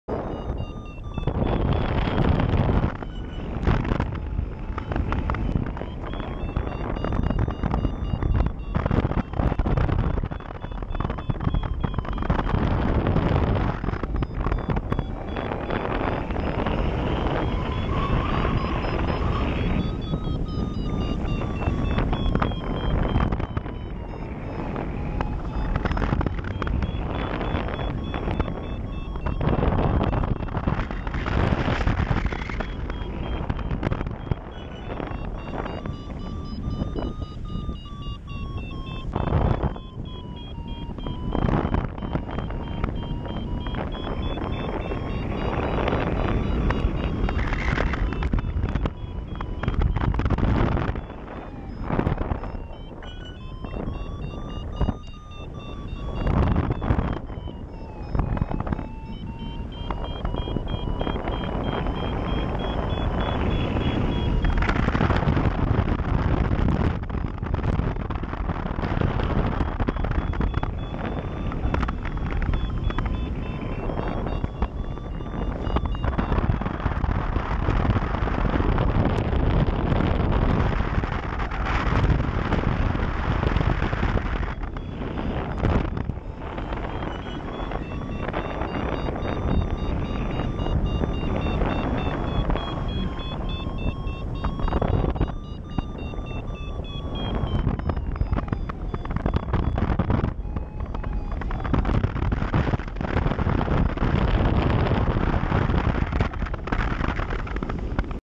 Paragliding, Closecall 2
A beautiful spring morning, gently winding up in light lift with other paragliders and hang gliders. A misunderstanding resulted in quite a close call. Everything can go from good to bad quickly when you are in amongst other wings...